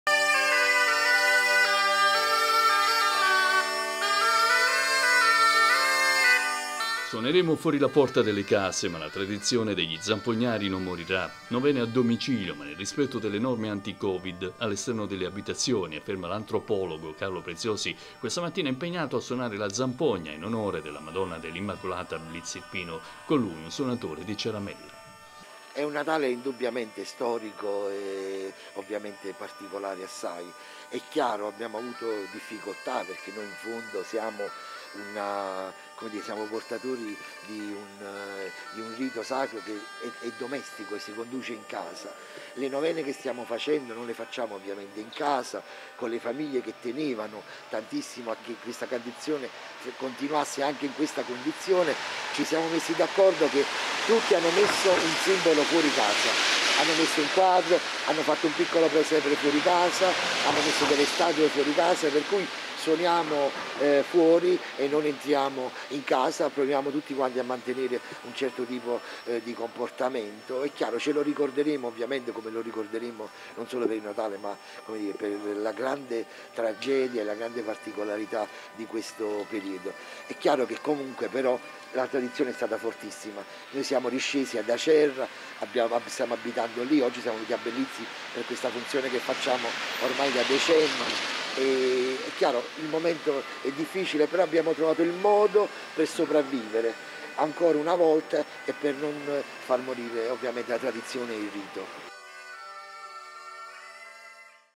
Il Covid non ferma la tradizione degli zampognari: “suoneremo le novene fuori la porta delle case”
Nel rispetto delle norme anti-covid gli zampognari suoneranno le novene fuori la porta delle case. Una tradizione che nonostante le difficoltà non può morire: commenta Carlo Preziosi.